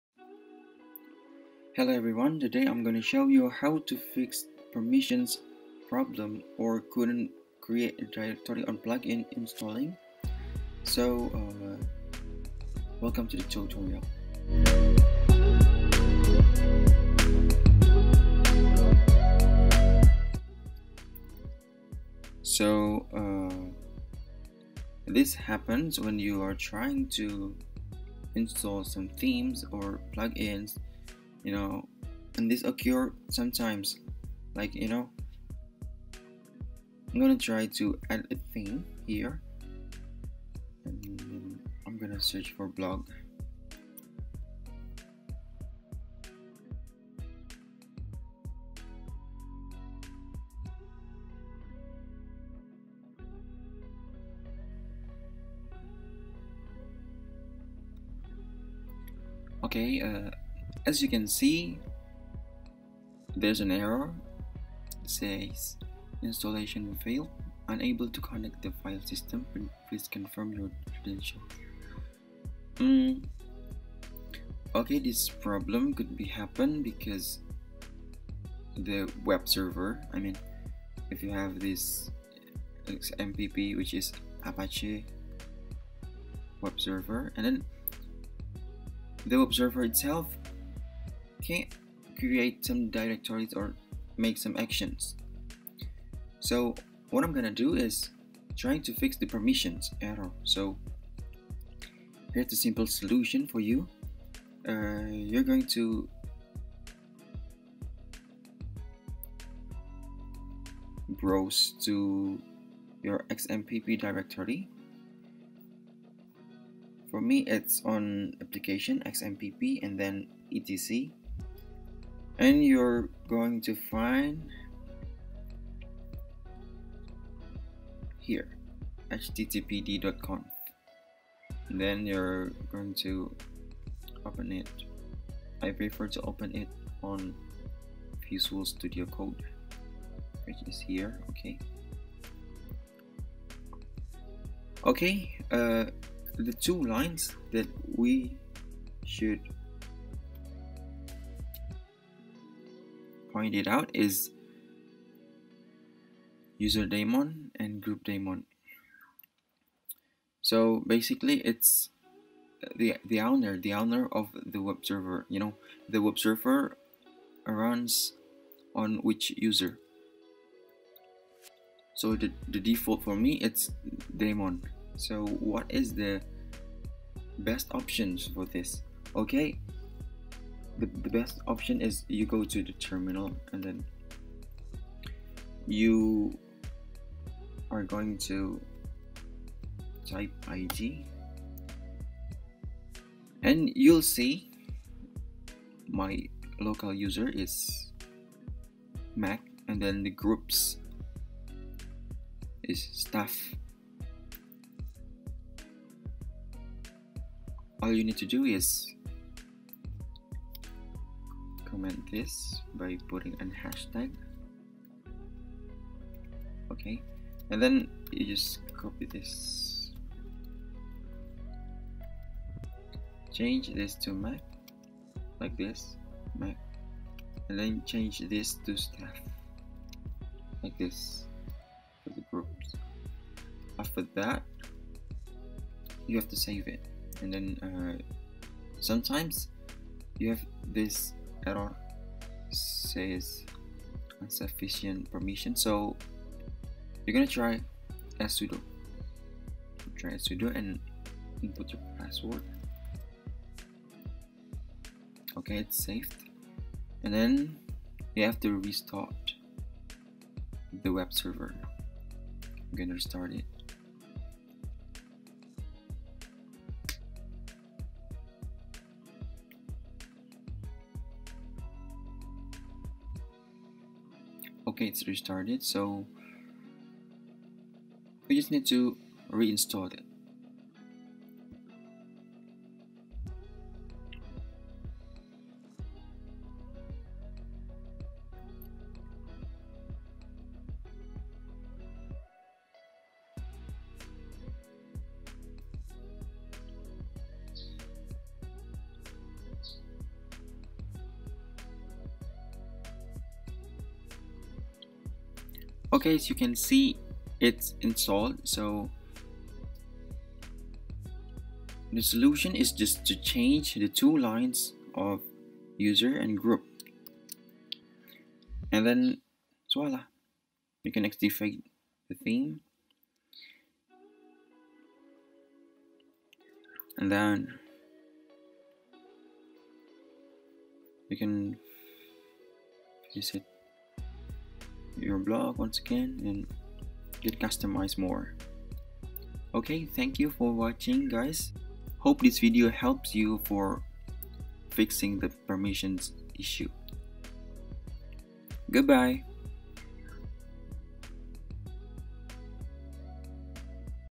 Fix Wordpress Permission (couldn’t install theme and plugin) on localhost Environment mac 2021
Today's video show us how to fix permission error that create: cannot install plugin, theme, or cannot create directory. By following this tutorial, you can fix the permission error.